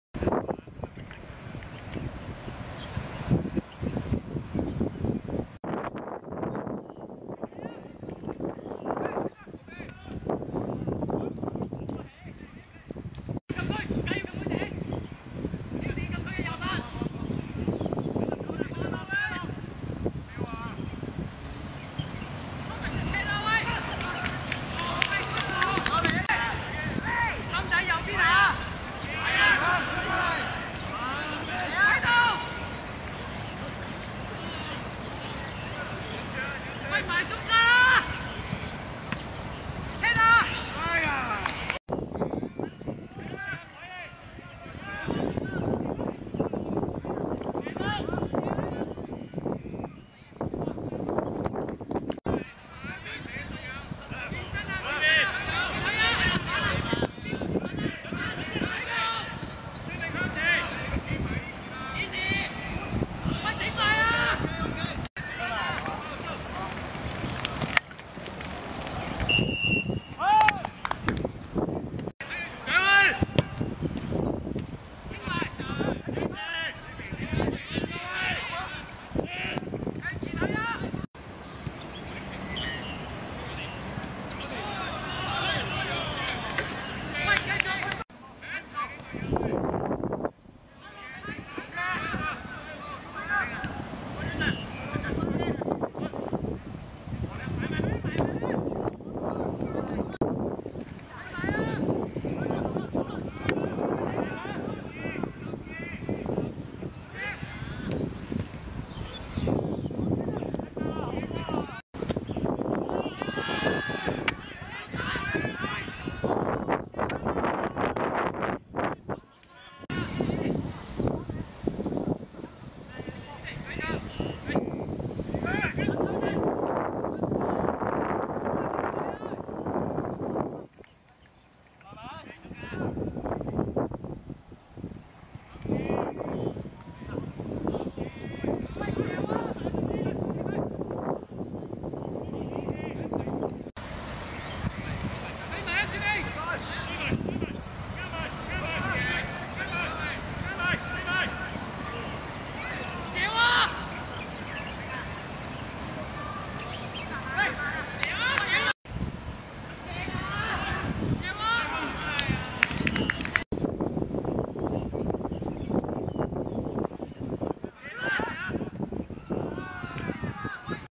A60vs趙俊傑邀請隊(2013Danehill六角賽挑戰盃A組)精華
2013.7.1.九龍仔仿真草塲 13:25 A60(黃) 0:0 趙俊傑邀請隊(紫)